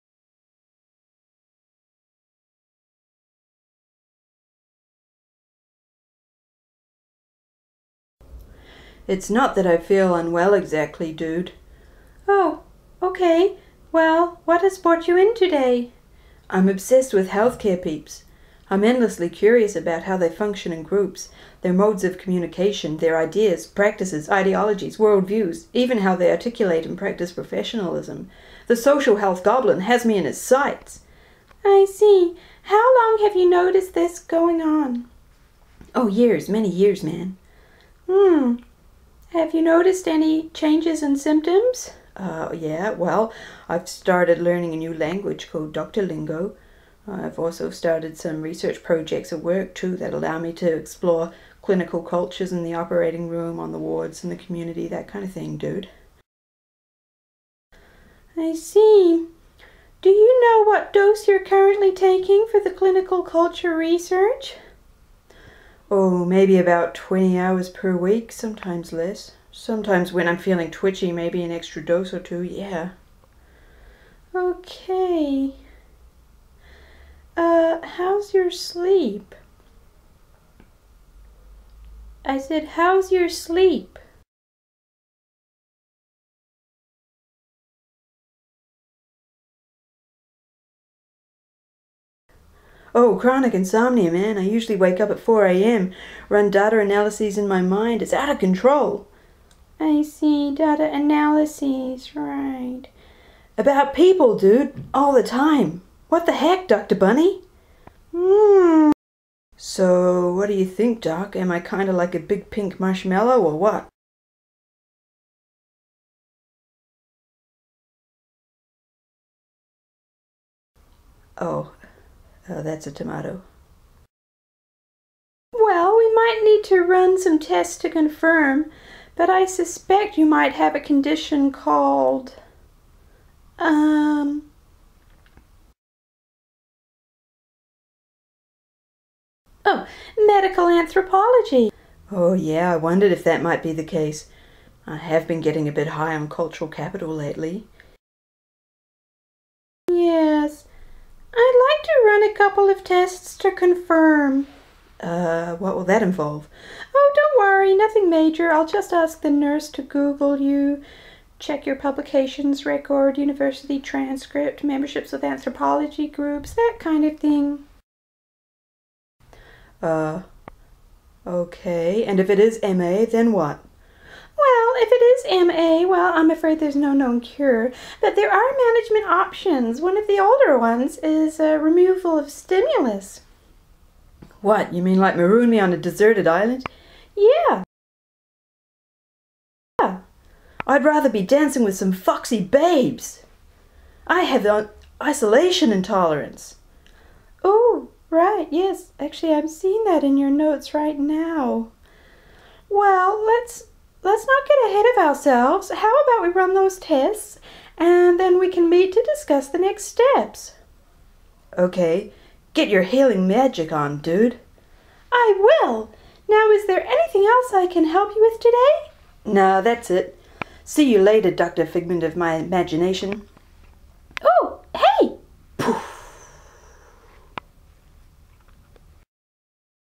Anthropology as illness May 2016
In this patient-doctor consult between Mr Fox and Dr Bunny the idea is posited that Mr Fox could be afflicted with an academic illness; medical anthropology. This is a humorous educational resource to introduce characteristics (symptoms) of anthropology.